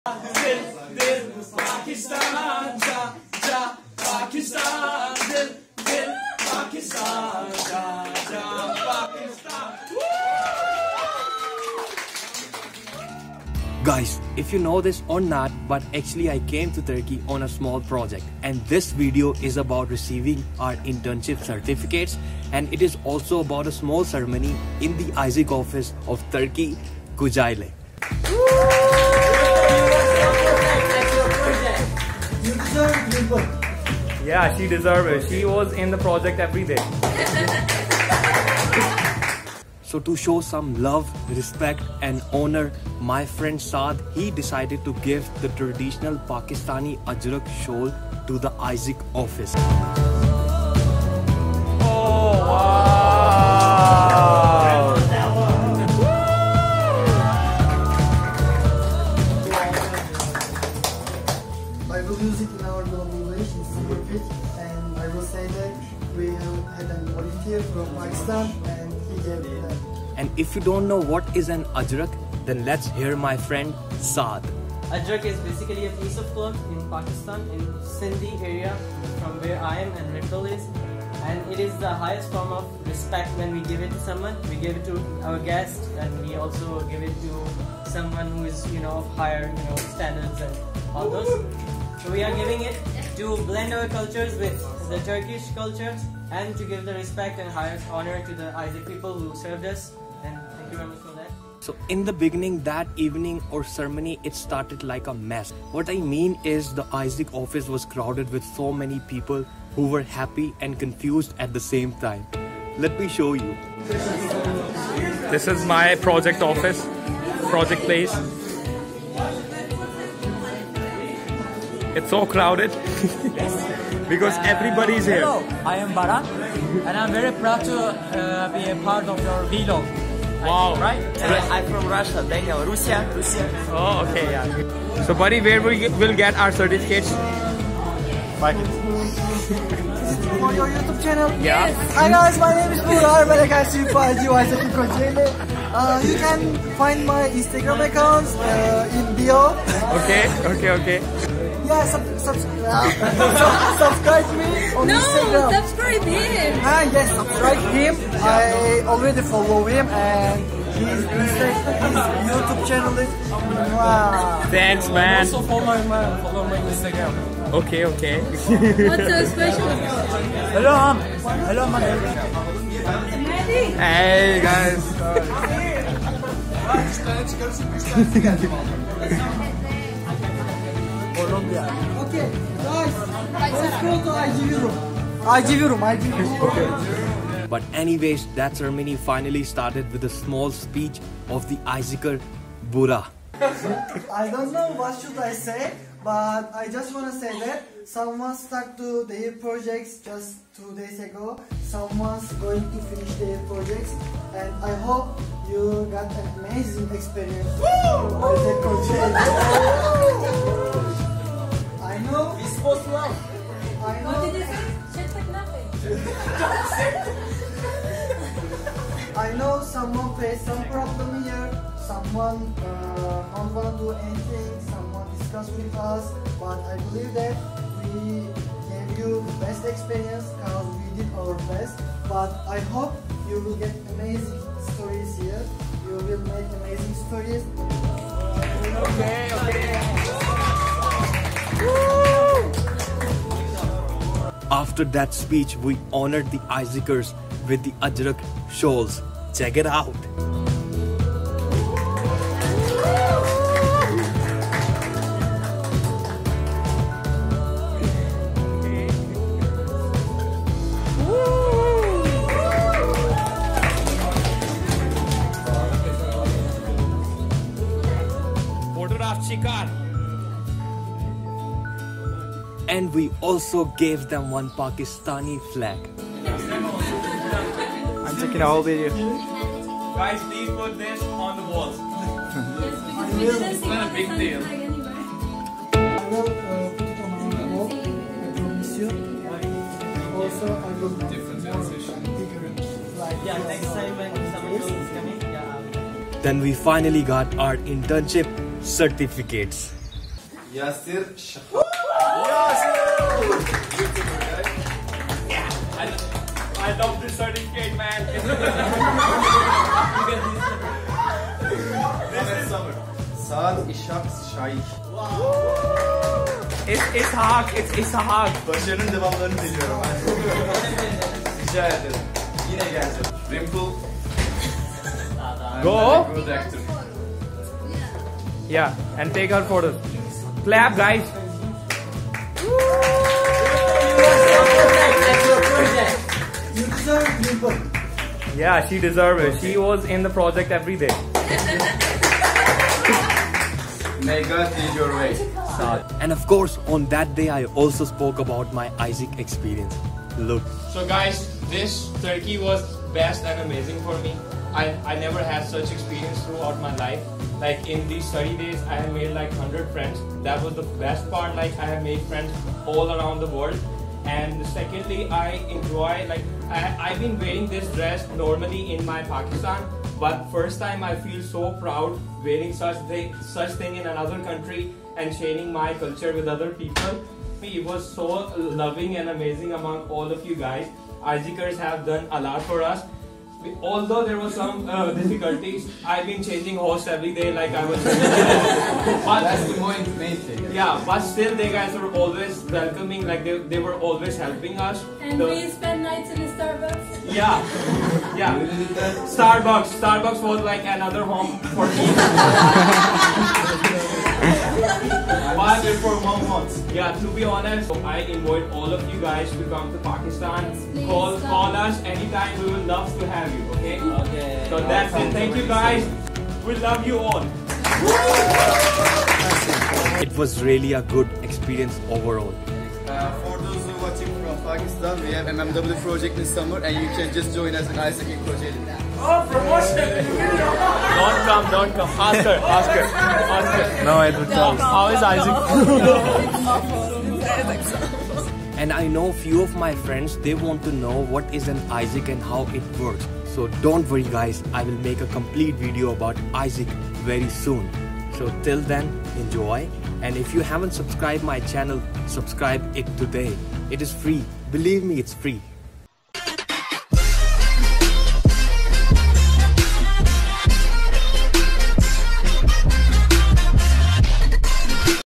Aiesec Turkey | Pakistani living in turkey | shor vlogs
My Aiesec experience in Turkey
being a Pakistani living in turkey | shor vlogs